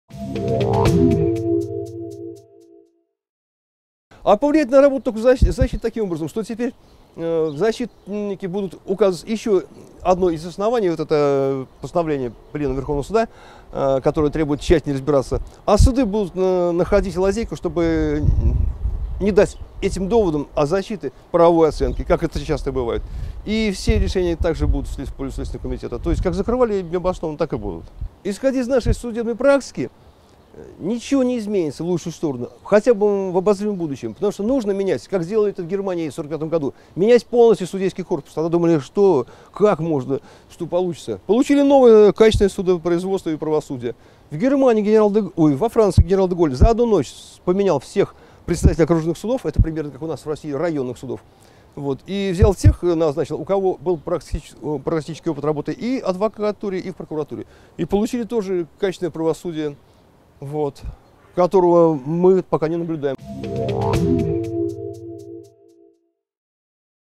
Комментарий. Виктор Синаюк (25.05.2016)
Верховный суд призвал отказаться от формального подхода при арестах. Руководитель областного правозащитного центра «Твое право» рассказал, как это повлияет на работу защиты.